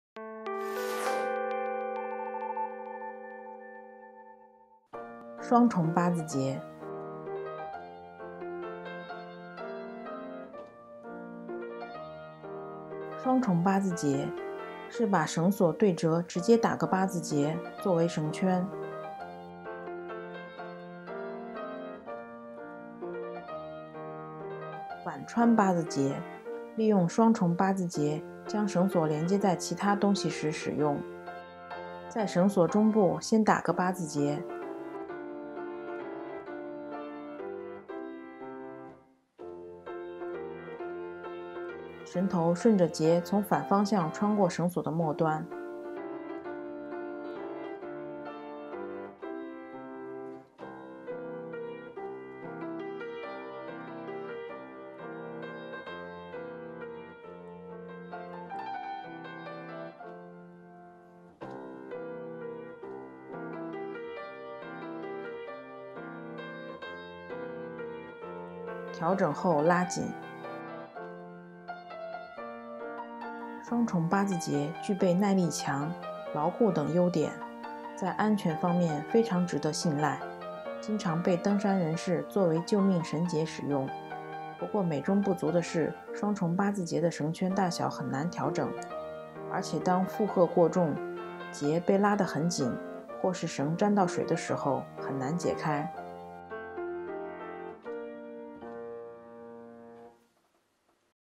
014双重八字结 How to tie a Double Figuer-eight Knot
双重八字结(Double Figuer-eight Knot)
双重八字结是把绳索对折直接打个八字结，作成绳圈。
反穿八字结，利用双重八字结将绳索连结在其它东西时使用。
在绳索中部先打个八字结，绳头顺着结从反方向穿过绳索的末端，调整后拉紧。

一日一结小常识：
双重八字结具备耐力强、牢固等优点，在安全方面非常值得信赖；经常被登山人士作为救命绳结使用。不过美中不足的是双重八字结的绳圈大小很难调整，而且当负荷过重，结被拉得很紧，或是绳沾到水的时候，很难解开。